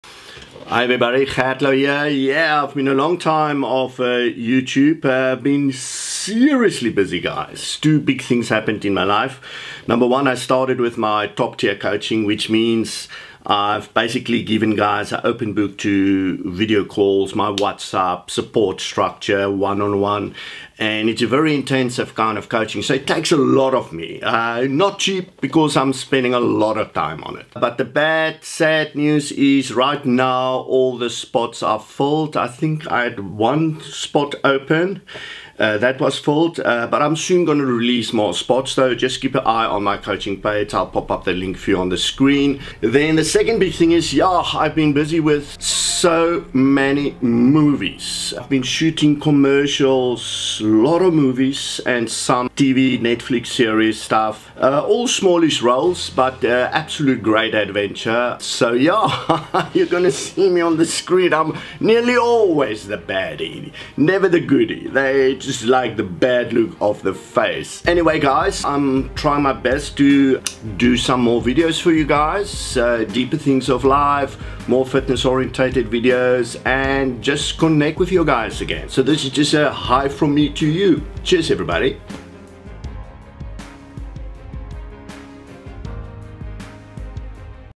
Living your BEST life - no matter your age!
I've been very busy this side but centering my balance and continuing with my youtube videos on fitness, health, life and more.
Been a crazy period behind me of kicking off with my TOP TIER COACHING as well as starting my limited but exciting acting career. Somehow cast just always as the baddie!

Happy training! Cheers...
Gert Louw